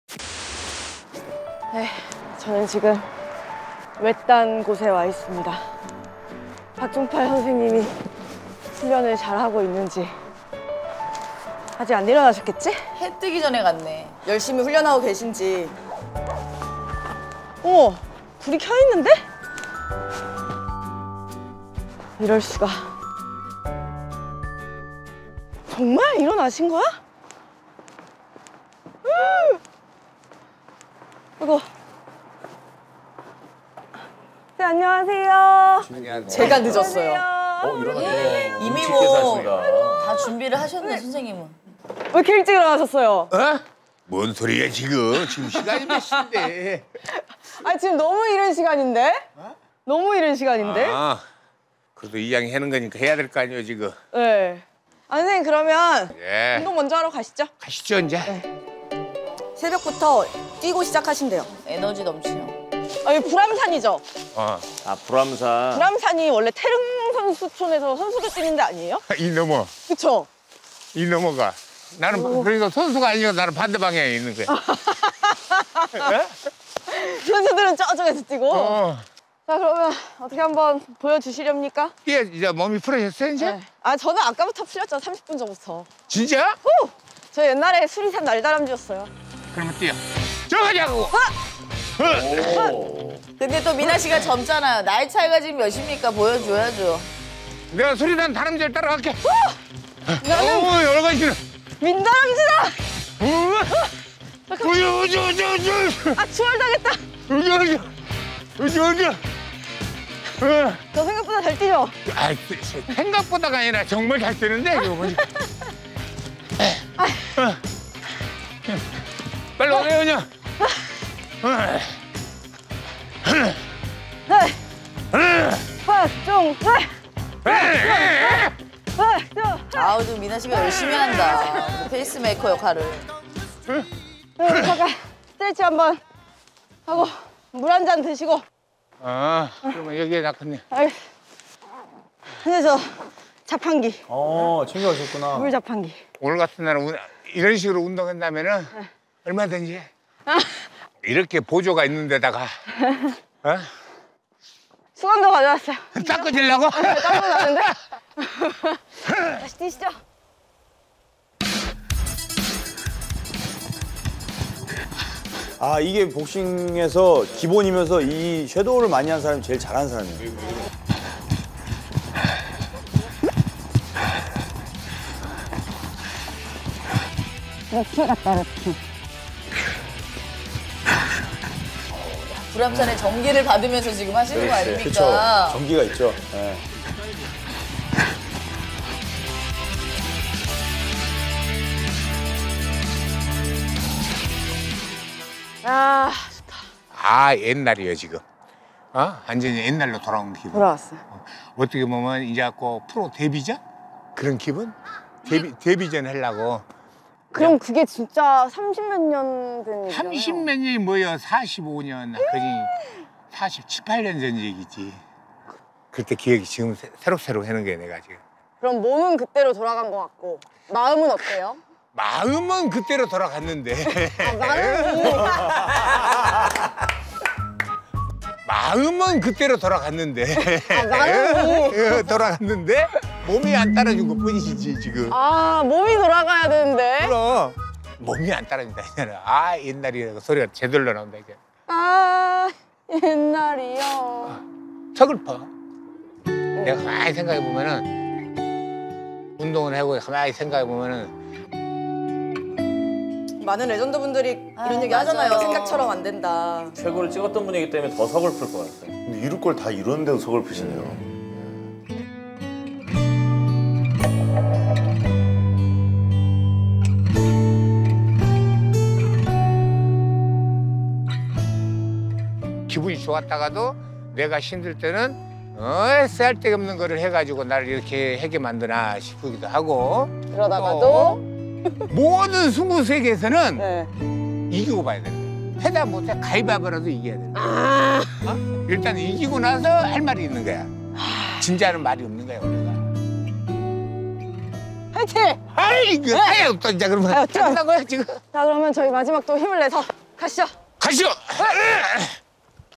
＂서글퍼…＂ 모닝 훈련 후 털어놓는 박종팔의 속마음 MBN 220423 방송
＂서글퍼…＂ 모닝 훈련 후 털어놓는 박종팔의 속마음